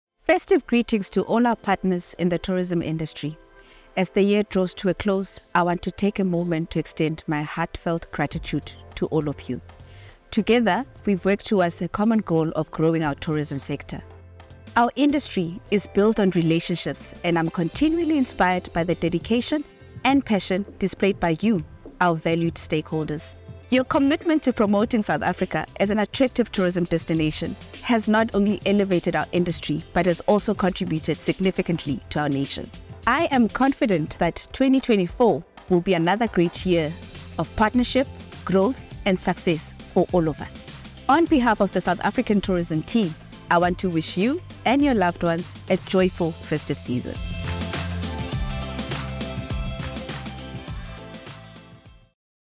South African Tourism Festive Season's Greeting
Festive greetings to all our partners in the tourism industry.
As the year draws to a close, the South African Tourism team wishes you and your loved ones a joyful festive season.